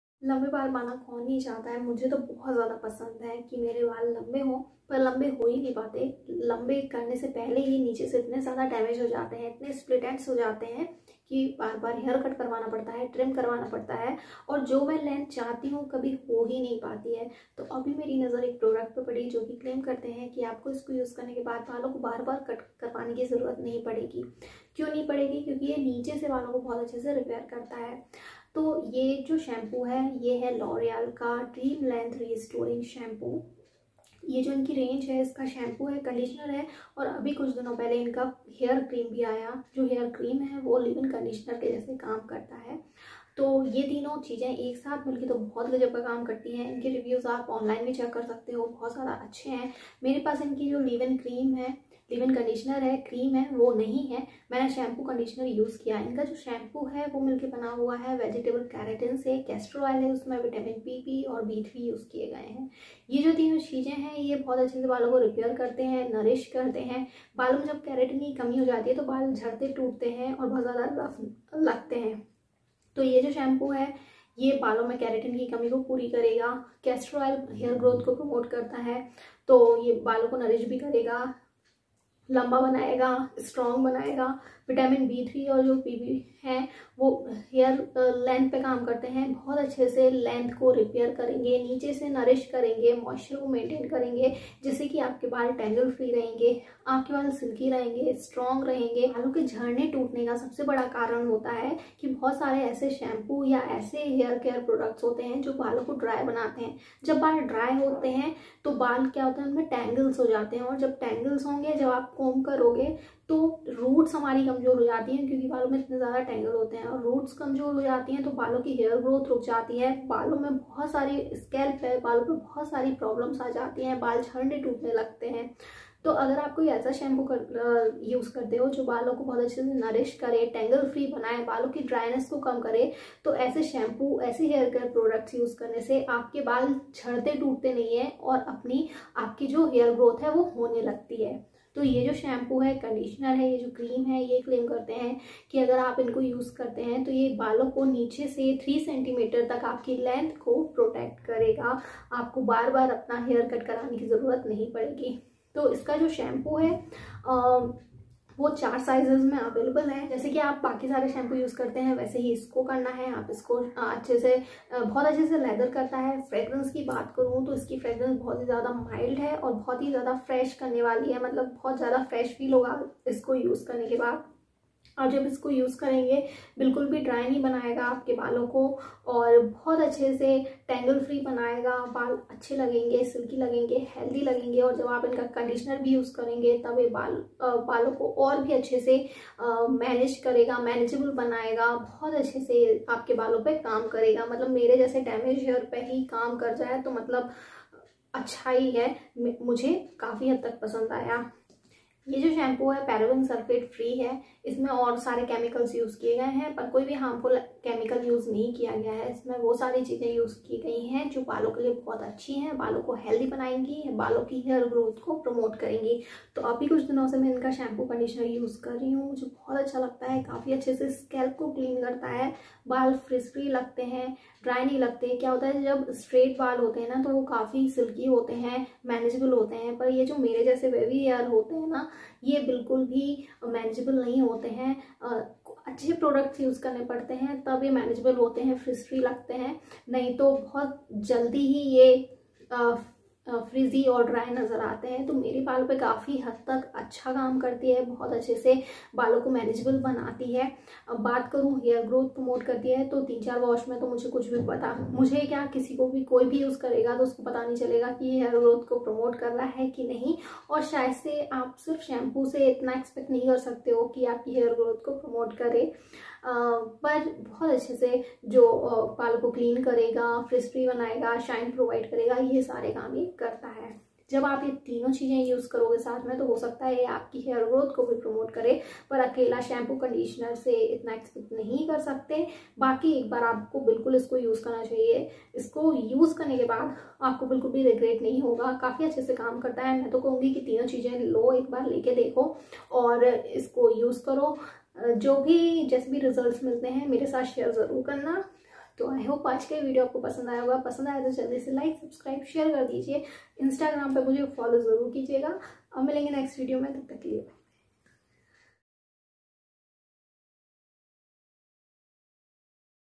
🟥LOREAL PARIS DREAM LENGTH SHAMPOO CONDITIONER & HAIR CREAM REVIEW/ बार बार बाल कटवाने की जरूरत नही
L'Oréal Paris Dream Lengths No Haircut Cream (Leave-In Conditioner)  (200 ml)

Hii Guys🙋
Is video me Loreal dreamlength restoring shampoo ka review kiya hai jo ki paraben sulphate free hai vegan hai and vegetable keratin,castor oil and vitamin pp and b3 se milkar bna hai.
ye shampoo apki length ko 3 cm tk protect krta khai.
balo ko tangle free bnaega,dryness km karega ,strong bnaega and hair growth ko promote krta hai.
ye range ke 3 products hai shampoo,conditioner and hair cfream ye teeno sath me milkar bht achha kaam krte hai.
aap ek baar jaroor try kare.
 ye shampoo 4 sizes me available hai .
aap purchase krna chahte hai to is link ko follow kare

L'Oréal Paris Dream Lengths No Haircut Cream Leave-In Conditioner, 200ml

thanks😍
Email- (business and collab)

l'oreal paris dream lengths
l'oreal paris dream length

l'oreal paris dream lengths no haircut cream
l'oreal paris cream